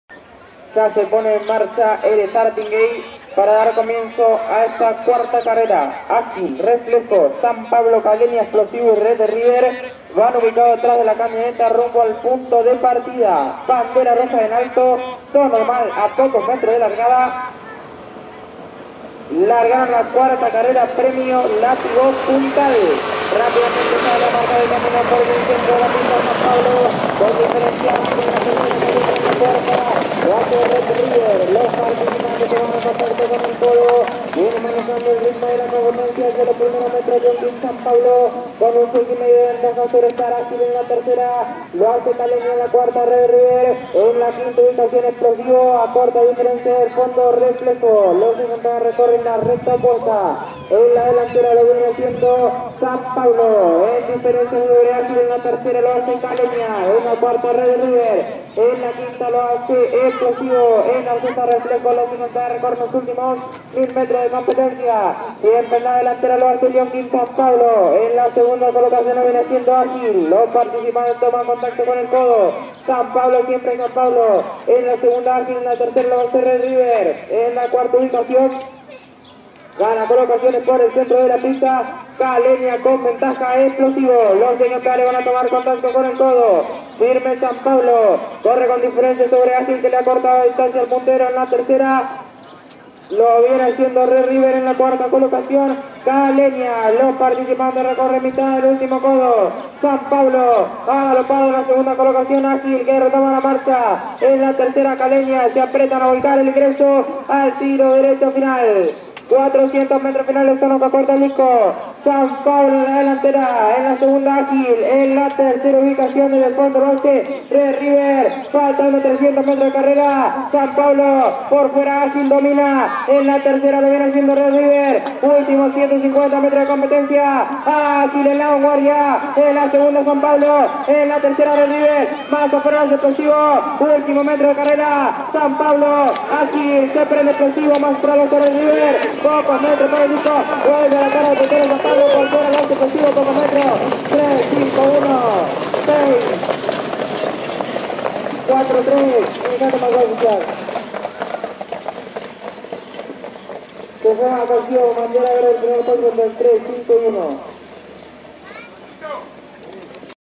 Hipodromo De Hurlingham - Reunion N° 31 4-09-2016 - carrera N° 4 -
Hipodromo De Hurlingham - Reunion N° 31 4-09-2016 - carrera N° 4 - Categoría: Productos Ganadores - Ejemplar: Lion King San Pablo - Driver: Miguel A. Castro - Distancia: 1609 Mts. - Tiempo: 2'04''64 - Promedio: 1'17''4 - Pista Normal.